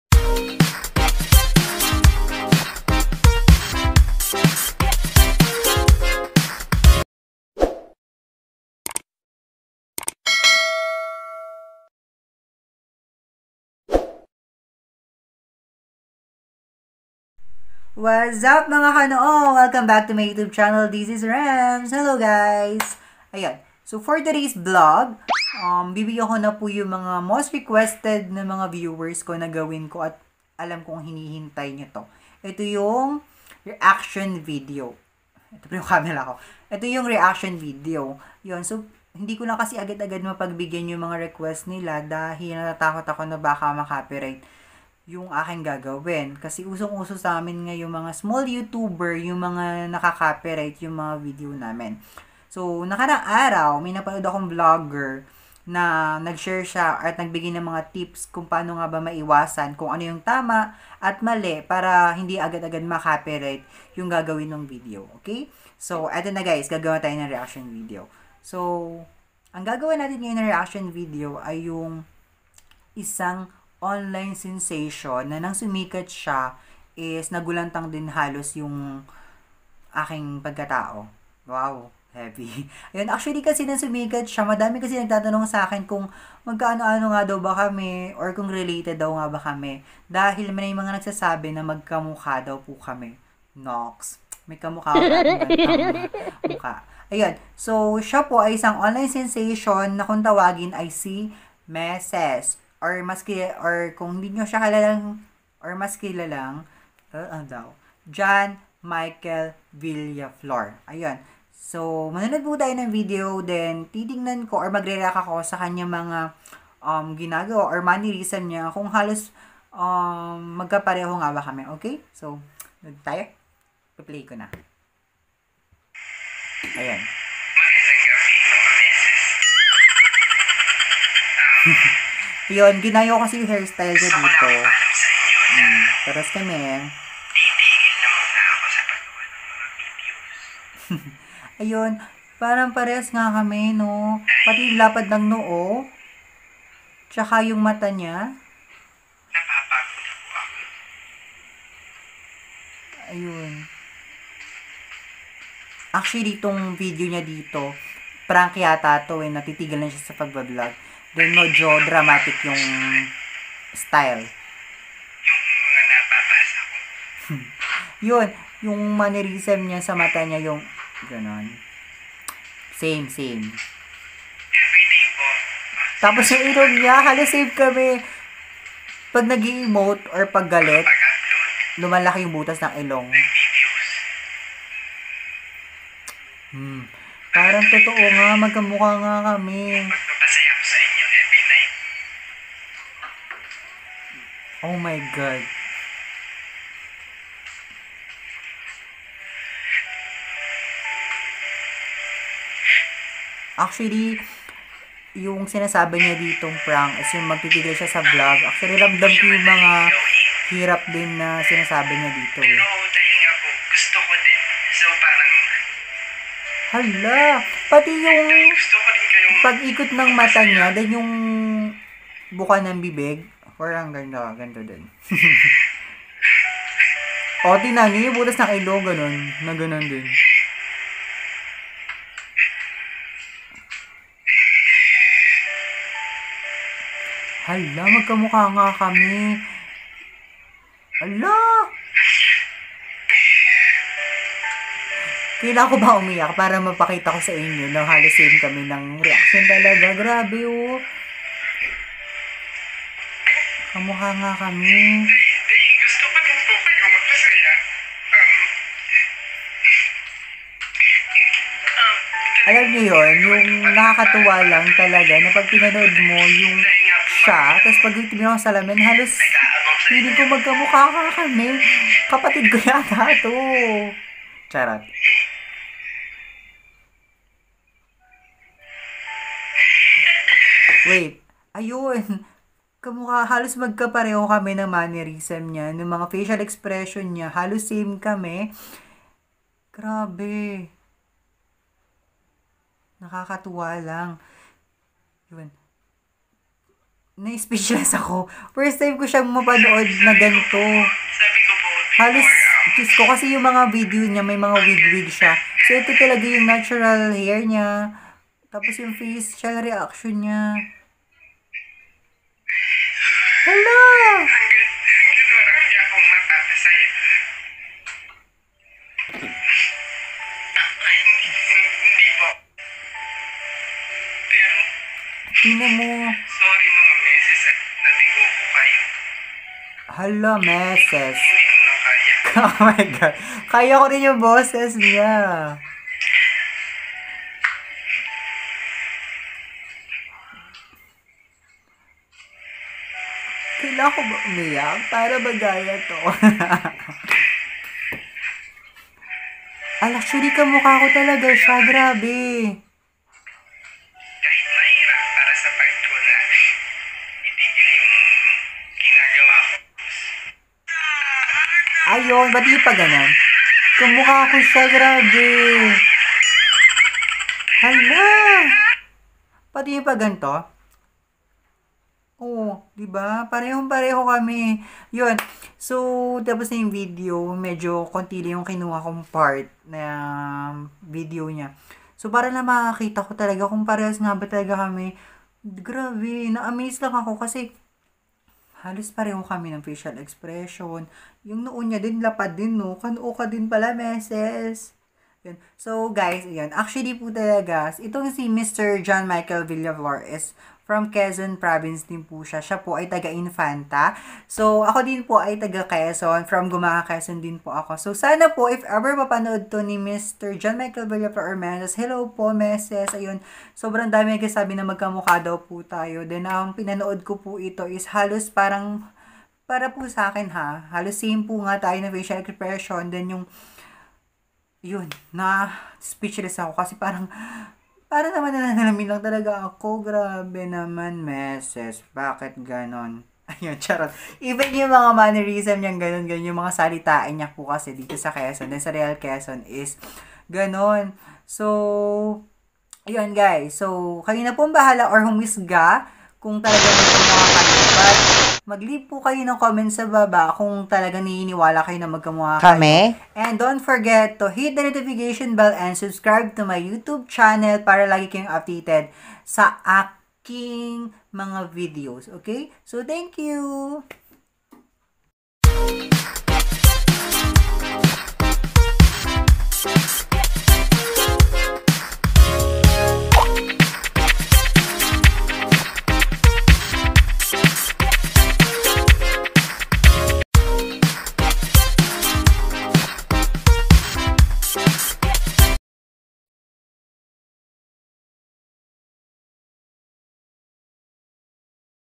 REACTION VIDEO KAY MESES NI NOOHAN MAGKAMUKHANGABA?
Ito na po ang isa sa mga request niyo ang gumawa ng reaction video.  So i hope na di ma copyright hahah weg nemen sene.

since lagi ako sinasabihan na mag kamukha kami ng Sikat na online vlogger na Si Meses ay ito na po ako na mismo nag react if kung totoo nga ba at nagulat ako na halos may same nga kami.

Leave lang po kayo ng comment sa babab if oo o hindi